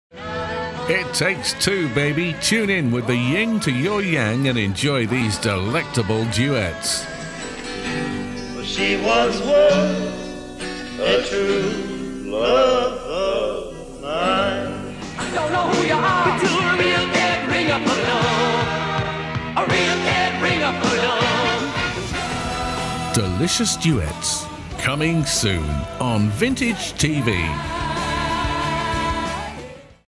Delicious Duets
Some of the most memorable duets in history, as Robbie meets Kylie, Macca mixes with Jacko, Dylan doubles up with Cash, Lionel serenades Shania and much more. Two can play this game!